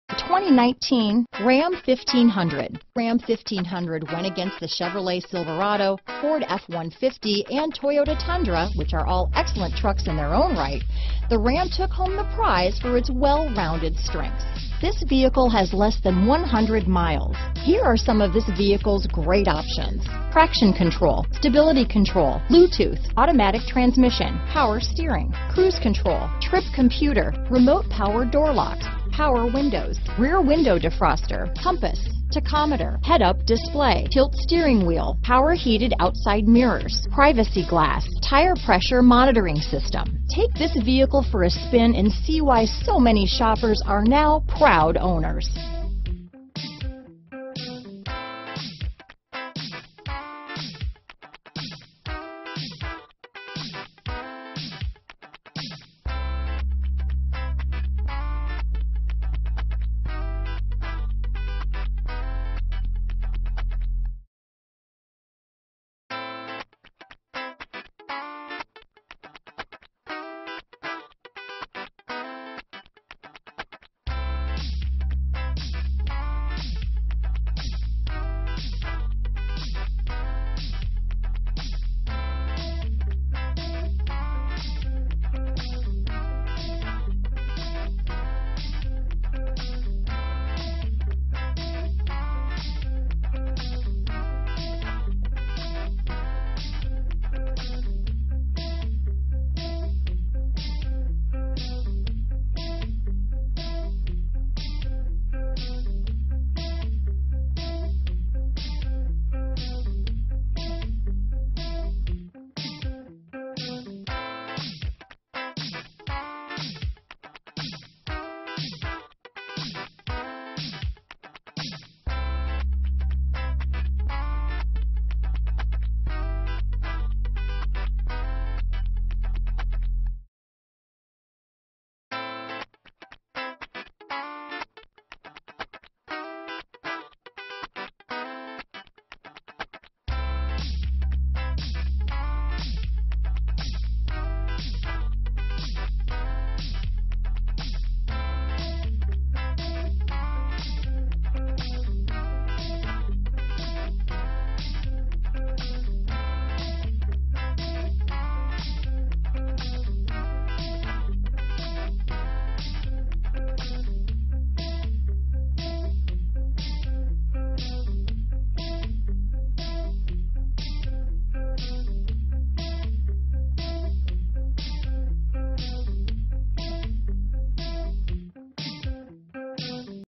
2019 RAM 1500 Classic Denver, Littleton, Aurora, Parker, Colorado Springs, CO R3589
Granite Crystal Metallic Clearcoat  2019 RAM 1500 Classic available in Denver Colorado at Brandon Dodge. Servicing the Littleton, Aurora, Parker, Colorado Springs, CO area.
2019 RAM 1500 Classic SLT - Stock#: R3589 - VIN#: 1C6RR7LT7KS629684

Brandon Dodge
5600 S Broadway